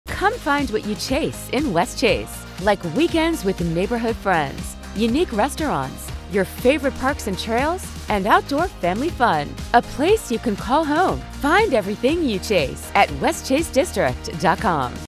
Live in Westchase District
Discover your ideal Houston apartment home, trails, family fun, and more! Experience a lively, secure community in Westchase District.

Find everything you chase at westchasedistrict.com